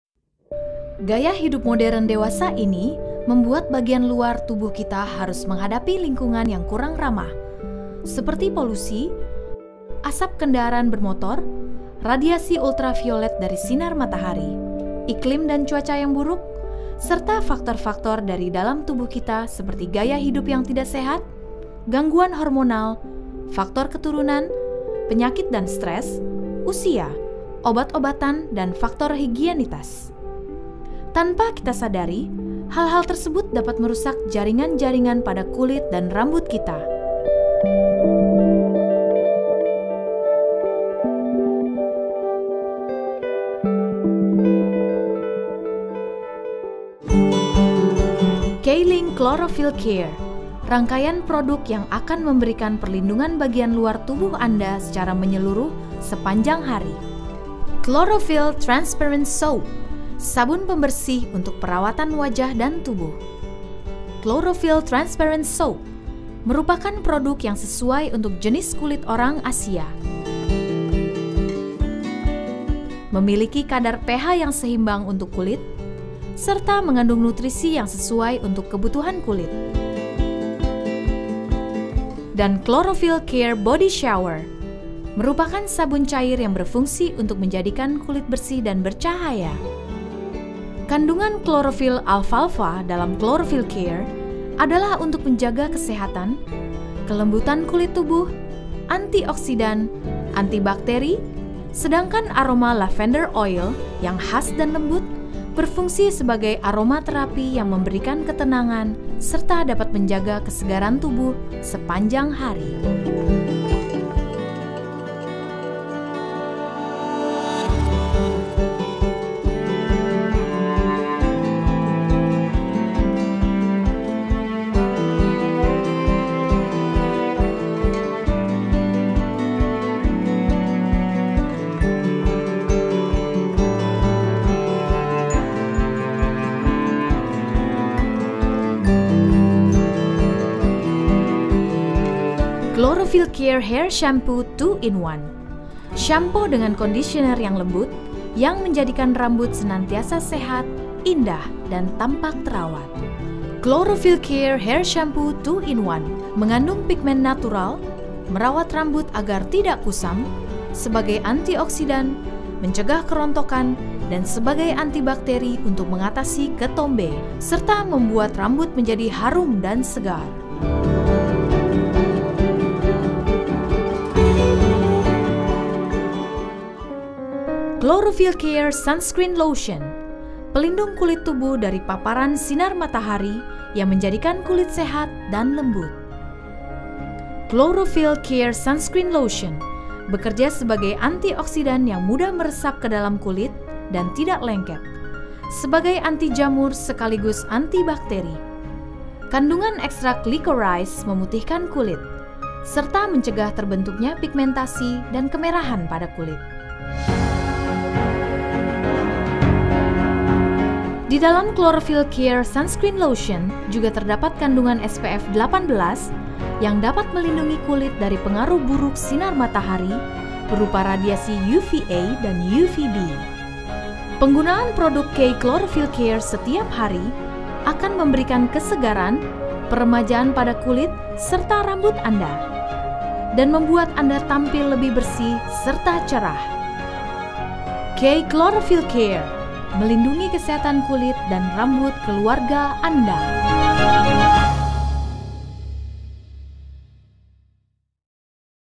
Clorophyl Care K-link
Transparant Soap.
     K-Care Chlorophyll Transparant Soap terbuat dari bahan-bahan khusus seperti pelembab, ekstrak/minyak tumbuhan serta vitamin atau nutrisi sesuai untuk kebutuhan kulit, sehingga kelembutan dan kesehatan kulit terpelihara. Dengan busa lembut dan sifat basanya yang lebih rendah, sabun ini tidak menimbulkan iritasi, kering dan keriput pada kulit.

Body Shower.
Manjakan tubuh anda saat mandi dengan K-Chlorophyll Care Body Shower. Rasakan busanya yang lembut serta aroma Lavender yang menenangkan. Menjadikan kulit bersih, bercahaya serta terjaga kelembutannya.

2 in 1 Shampoo.
K-Care Chlorophyll 2 in 1 Shampoo mengandung chlorophyll sebagai natural pigment yang dapat merawat rambut Anda agar tidak kusam. Dilengkapi dengan aroma Lavender yang khas, sangat cocok untuk digunakan setiap hari untuk menjaga kesegaran rambut.

Sunscreen Lotion
K-Care Chlorophyll Sunscreen Lotion dengan kandungan Chlorophyll Alfafa yang tidak lengket dan mudah meresap ke dalam kulit, menjadikan kulit begitu halus dan lembut. Kandungan aroma Lavender yang khas sangat cocok digunakan setiap hari untuk merawat dan menjaga kesegaran kulit. Diperkaya juga dengan Ekstrak Licorice, Ekstrak Bearberry sebagai bahan pemutih dan SPF untuk melindungi kulit dari efek buruk sinar matahari.

Manfaat :
•Menjaga dan mempertahankan kesehatan kulit.
•Mencegah kulit menjadi kusam, layu dan keriput.
•Menjaga kelembapan, kekenyalan dan kehalusan kulit, menstabilkan pH kulit serta membantu regenerasi sel kulit.
•Mencegah timbulnya jerawat.
•Dapat membunuh bakteri dan jamur.
•Aman digunakan oleh semua umur : untuk bayi, remaja, dewasa atau bagi usia lanjut.
•Dapat digunakan setiap hari sebagai sabun mandi yang aman untuk kulit.

Manfaat Sunscreen Lotion :
•Melindungi efek buruk sinar matahari.
•Memantulkan radiasi dari panjang gelombang ultraviolet.
•Memperkecil resiko kanker kulit.
•Memperlambat proses penuaan kulit dini (prematur skin aging)
•Aman untuk semua jenis kulit.
•Tidak mengandung PABA (Para Amino Benzoil Acid) yang dapat mencoklatkan kulit (tanning)
• Tidak menyebabkan jerawat ( non acnegenic)
• kandungan SPF 18
• Sunscreen lotion, anti air dan lebih tahan lama